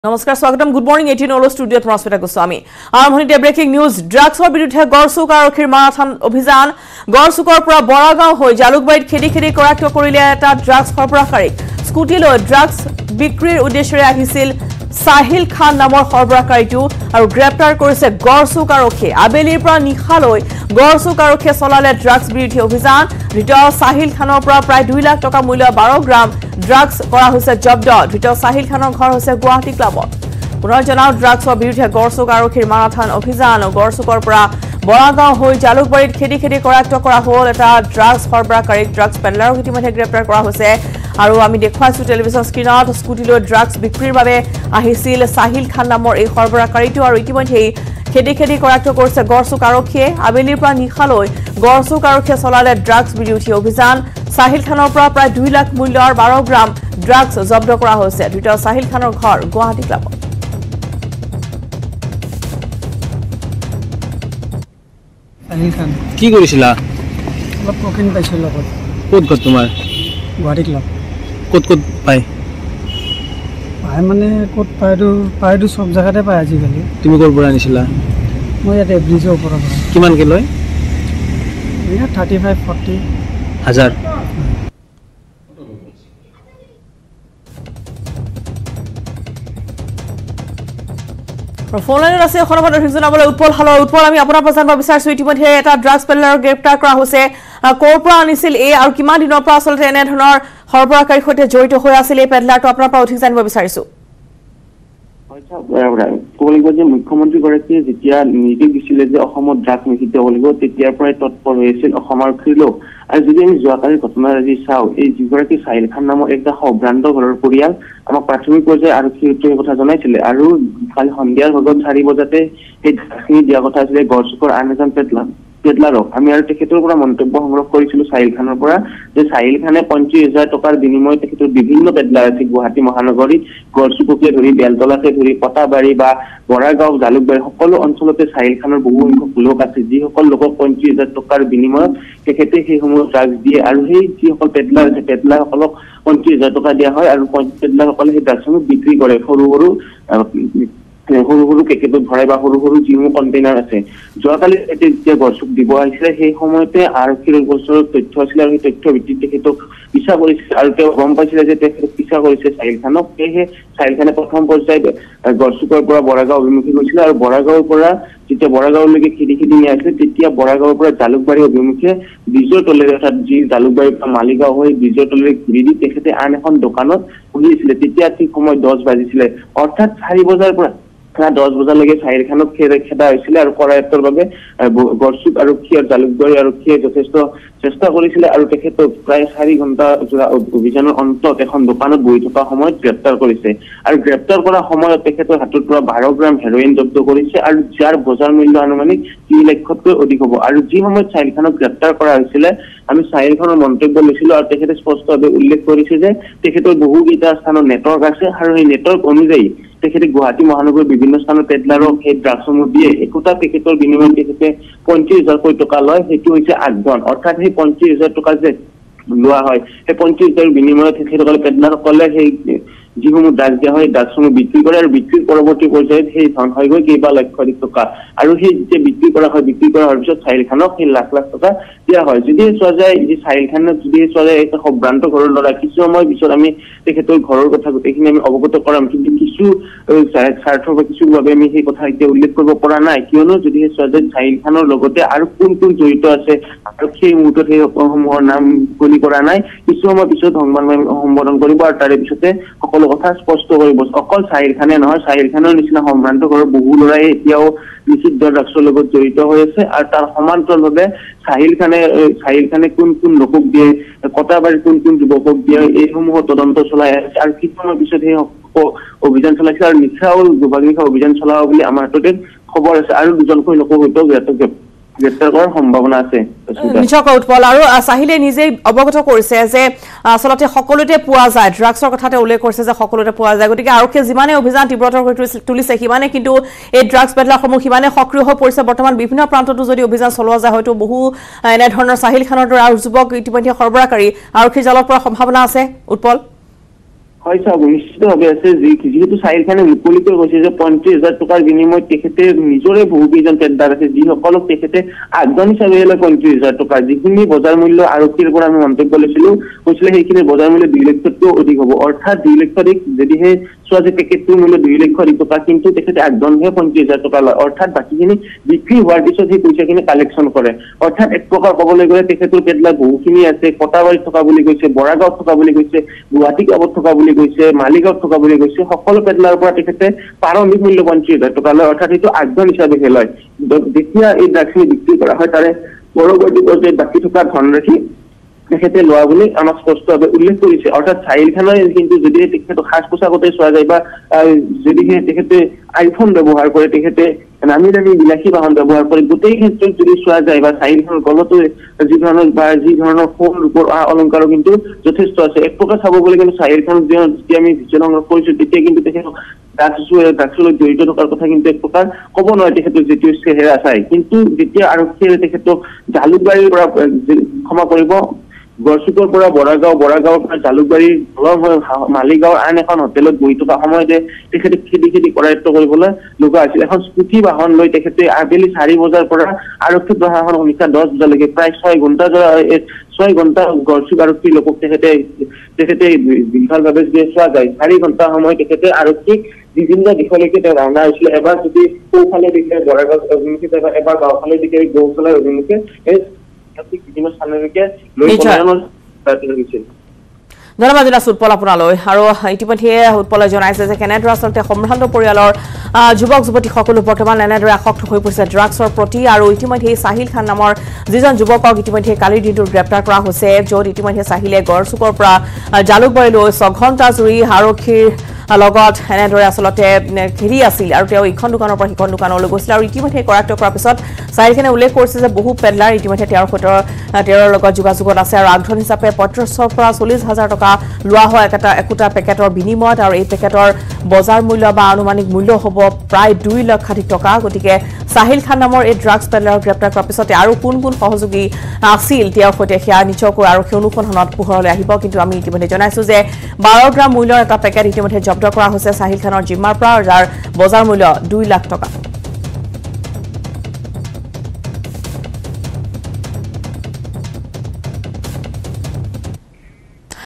Gorchuk Drugs News | গড়চুক আৰক্ষীৰ জালত ড্ৰাগছসহ সৰবৰাহকাৰী
7 November, 2021 : প্ৰায় ২লাখ টকা মূল্যৰ ১২গ্ৰাম ড্ৰাগছ জব্দ । আবেলিৰ পৰা নিশালৈকে গড়চুক আৰক্ষীয়ে চলাইছিল ড্ৰাগছ বিৰোধী অভিযান ।